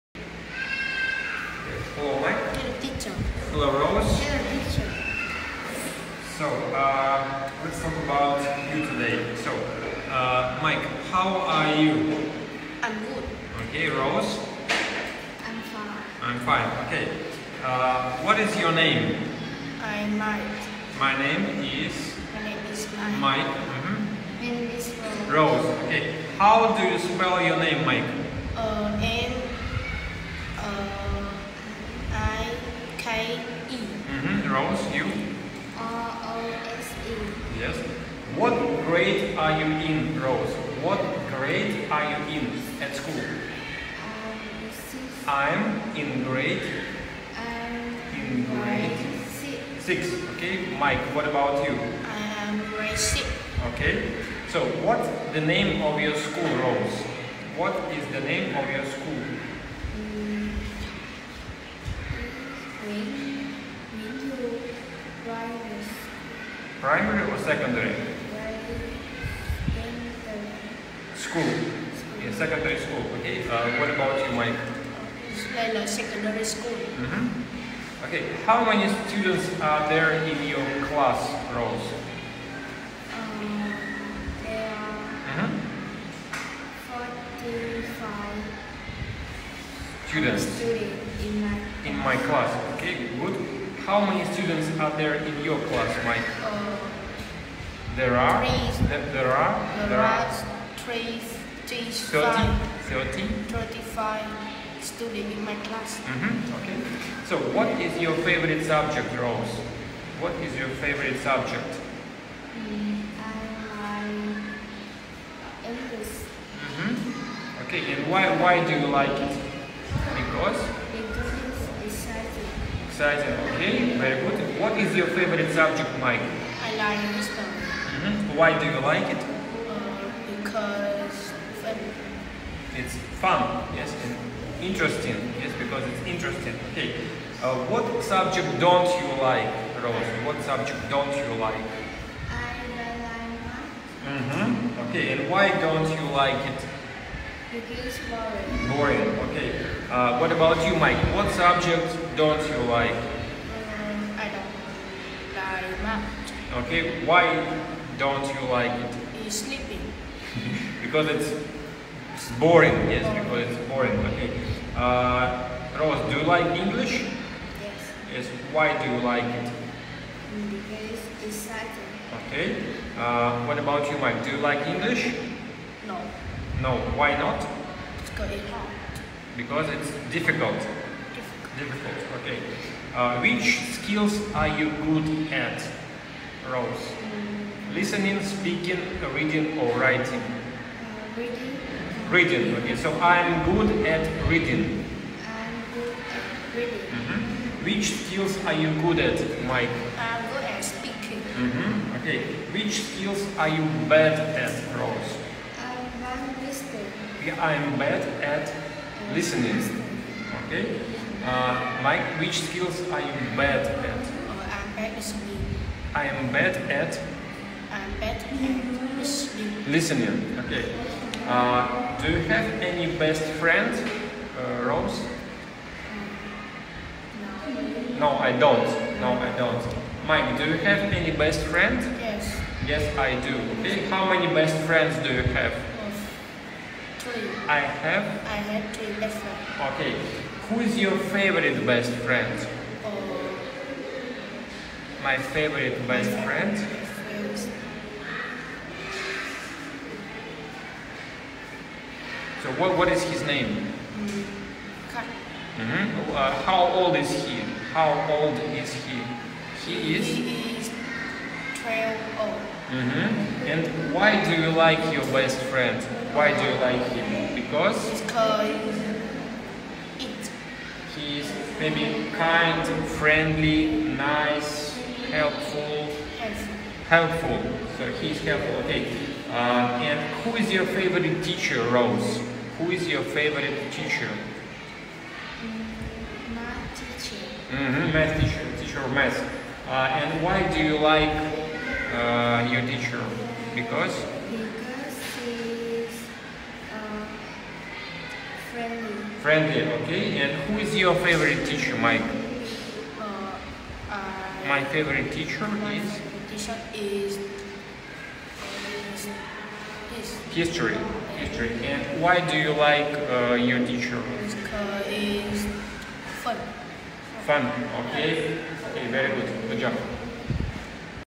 Tiếng Anh Giao Tiếp Thiếu Nhi Tại Phan Thiết: Mike and Rose talking about School and Study
Mike and Rose practice talking about School and Study by answering and asking questions with Michael!